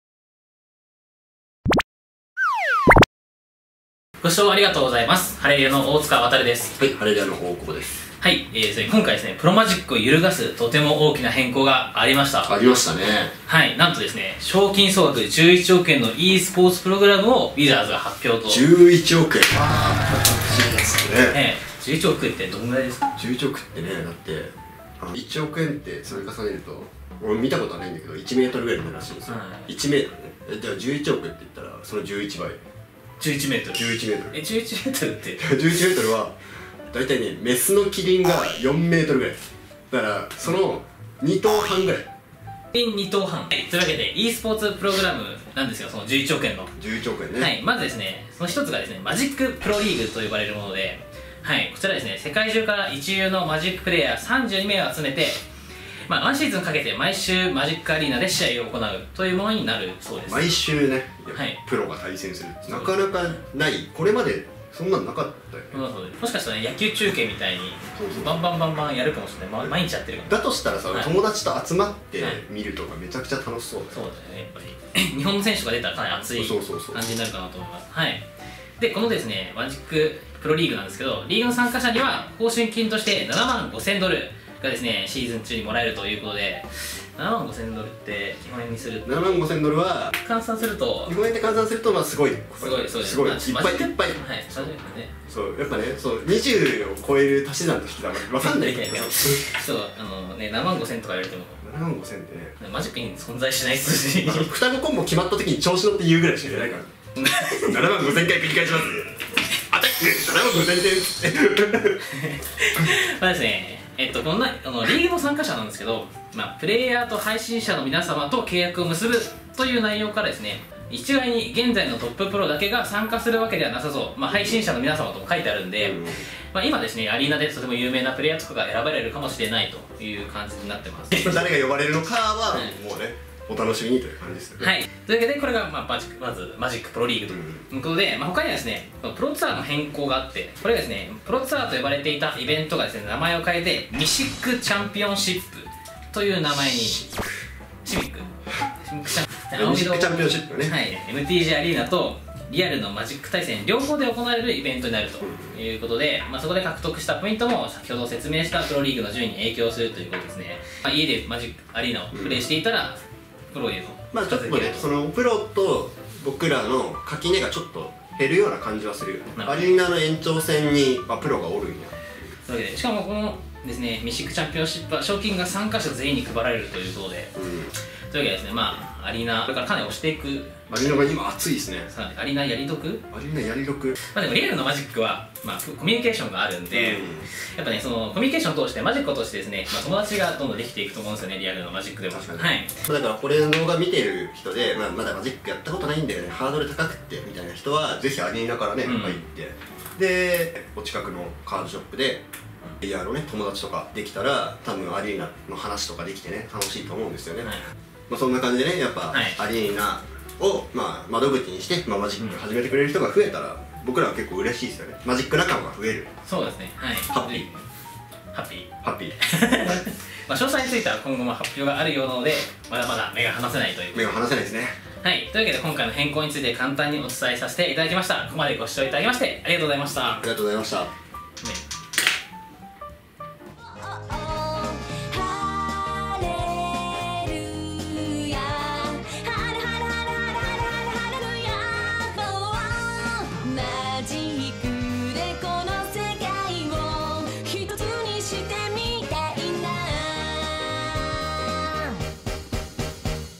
【MTG】賞金総額11億円!? MTGeスポーツプログラムとは？【晴れる屋】
12月6日発表されたMagicTheGatheringのeスポーツプログラムを
晴れる屋 広報の大久保 寛と大塚 航が語ります！